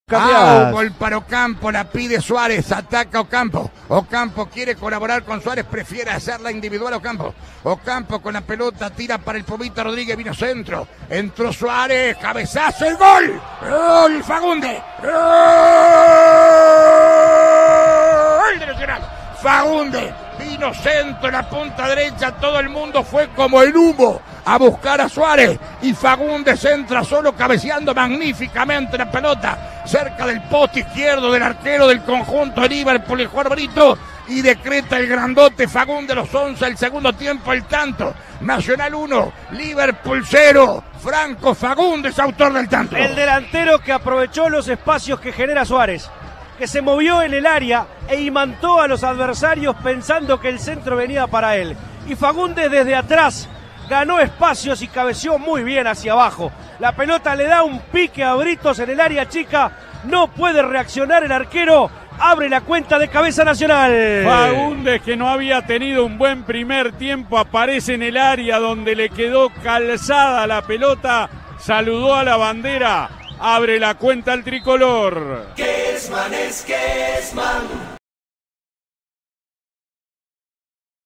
Liverpool 0 Nacional 1 - Clausura 2022 (por Alberto Kesman)
El gol del partido, por Alberto Kesman junto a Ernesto Faría y Richard Arce para CX 22 Radio Universal 970 AM.